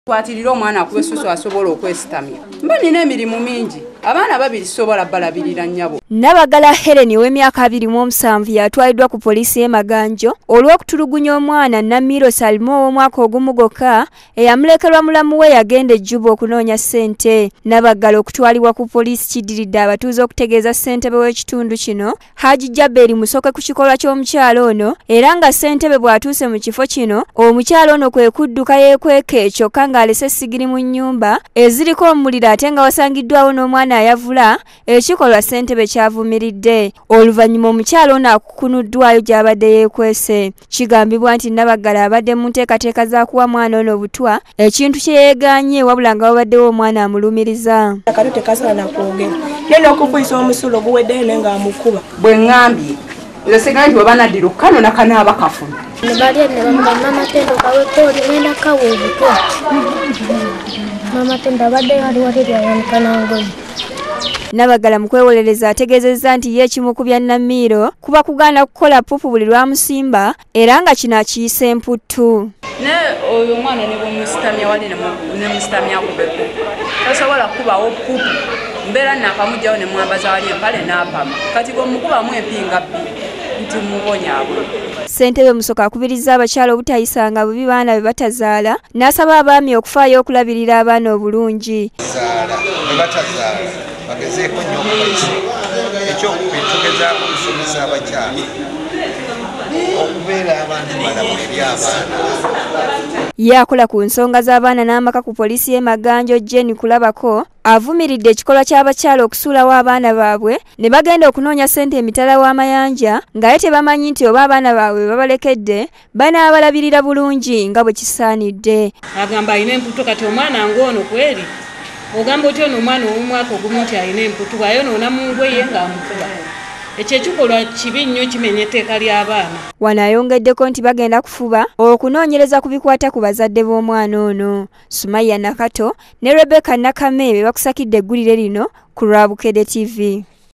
Omukazi akwatiddwa lwa kutulugunya mwana
Abadde agenda kumuwa butwa